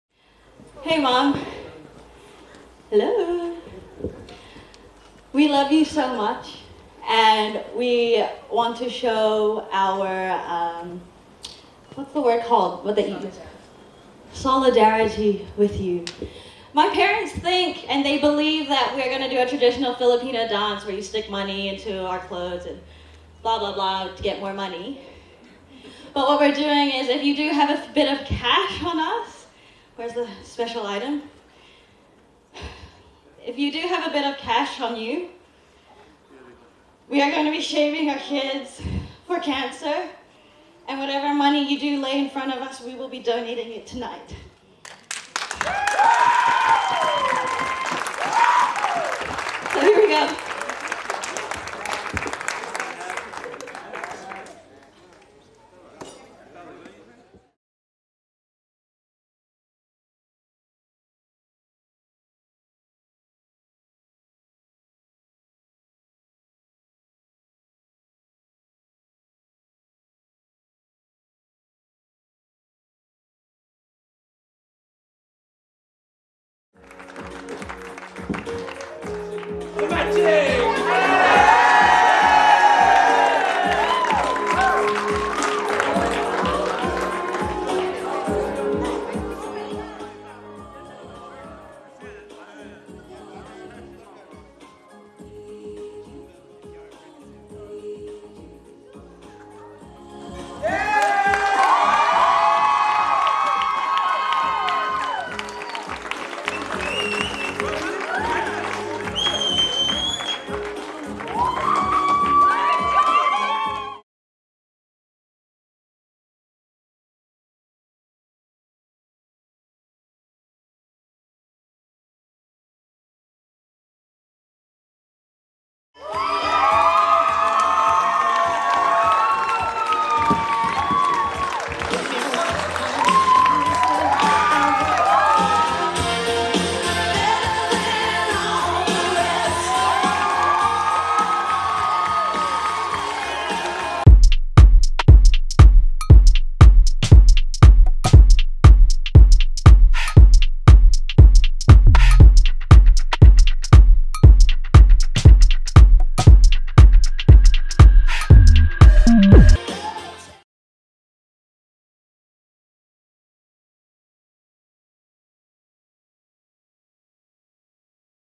شاهد .. زوجان يحلقان شعرهما يوم الزفاف أمام الضيوف لسبب غريب
أقدم زوجان جديدان على حلق شعر رأسهما أمام الضيوف في حفل زواجهما بدلا من الرقص سويا، وذلك تضامنا مع والدة العروس التي تحارب السرطان.